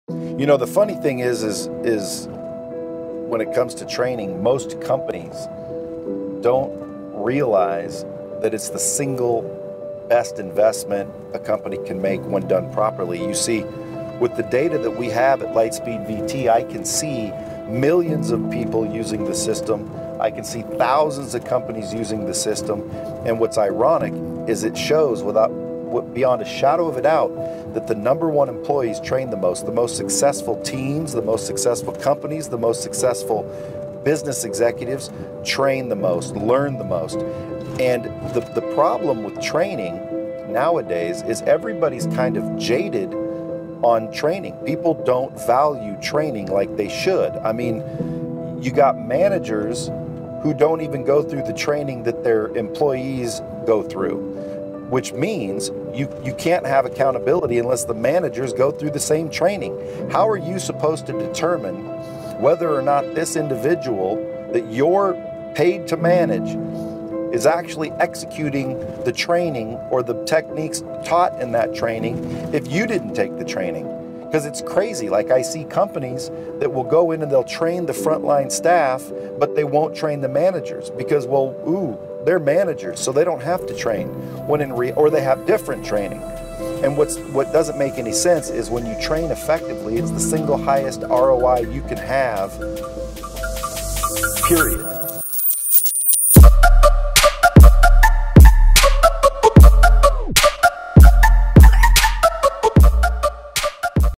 Sales Training = Best Investment
What's the best investment a company can make? hear it from a Sales Expert, Brad Lea.

Don't forget to Tag your manager 😅